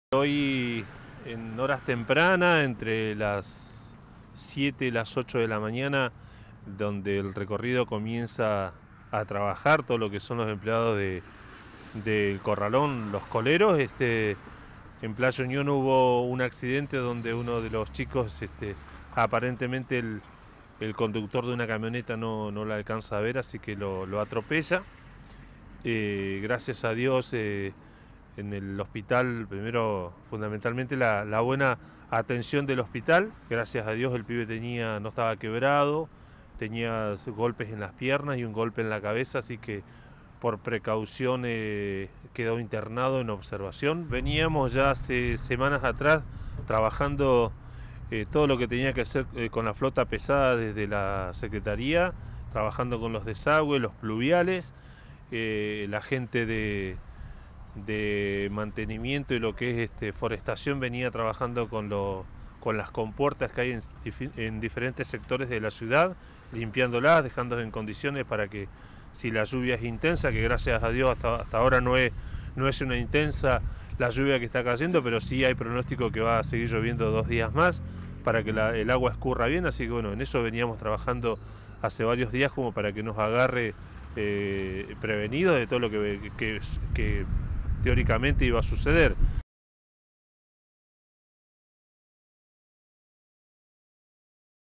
Recolector atropellado
Ayer a la mañana, un recolector fue atropellado mientras realizaba su trabajo en Playa Unión. Una camioneta lo llevó por delante provocándole lesiones. Fue derivado al hospital Santa Teresita y se recupera favorablemente. De esto hablamos con el secretario de Obras Públicas de Rawson, quien además habló de los trabajos que se realizan para que las lluvias no generen inconvenientes en la ciudad.
12 Noticias